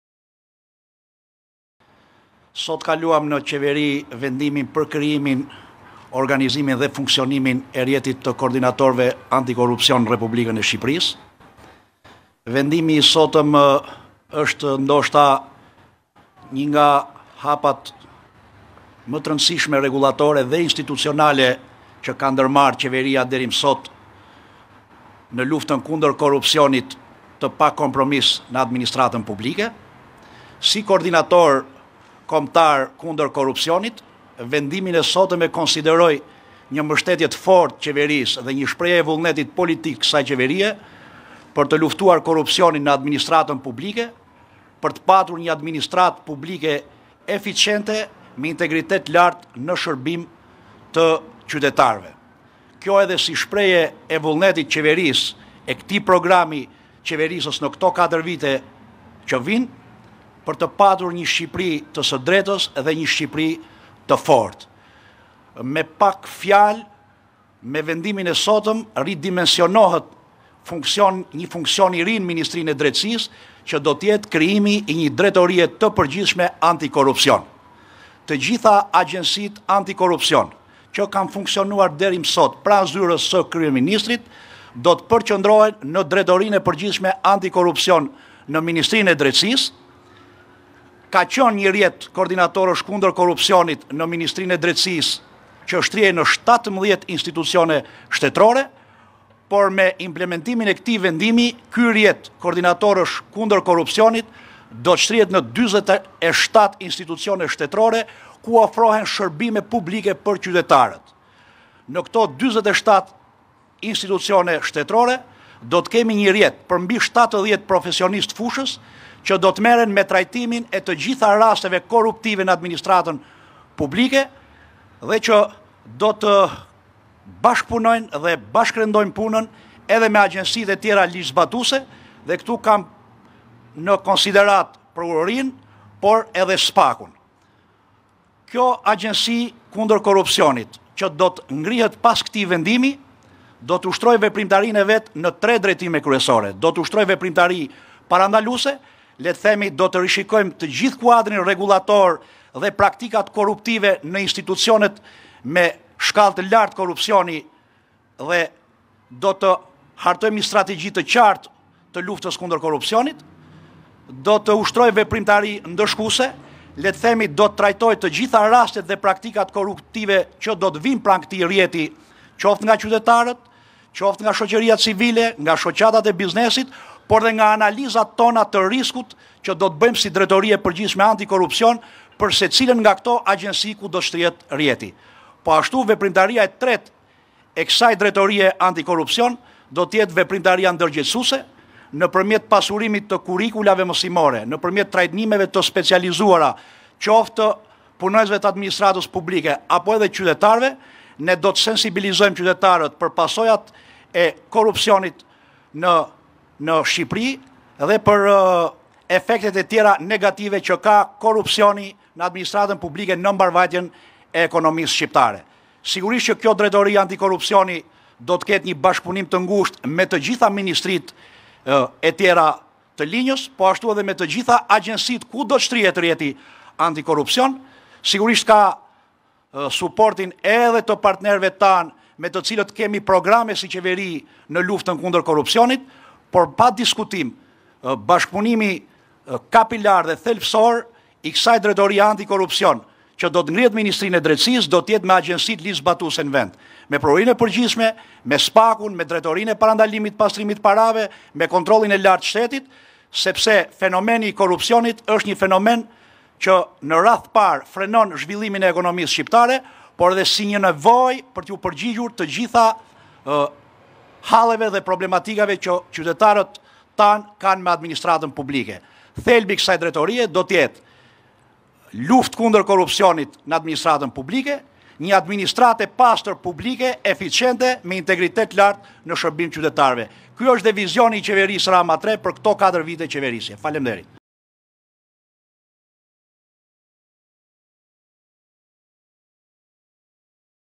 Drejtoria e re antikorrupsion, Manja: Do shtrihet në 47 institucione!
Manja: Sot kaluam në qeveri vendimin për krijimin organizimin e rrjetetit të kordinatorëve të rrjetit antikorrupsion në Shqipëri. kjo është një nga hapat më të rëndëisishëm që ka ndërmarrë Shqipëria deri më sot. Vendimin e konsideroj si vullnet të qeverisë për të pasur një administratë eficente. Si shprehje e vullnetit të qeverisë, për të pasur një vend të së drejtës. Me vendimin e sotëm ridimensionohet një funksion i ri në ministrinë e Drejtësisë. Ky rrjet kordinatorësh kundër korrupsionit do të shtrihet në 47 institucione shtetëore, do ketë rrjet për profesionistë të fushës që do merren me rastet korruptive. Kjo agjensi kundër korrupsionit, do ushtrojë veprimtari parandaluese, do të hartojë strategji të qartë, veprimtari ndëshkuese, e treta do jetë ndërgjegjësuese, duke kryer trajnime të specializuara. Do sensibilizojmë qytetarët në vend. Thelbi do jetë një administratë e pastër dhe eficente.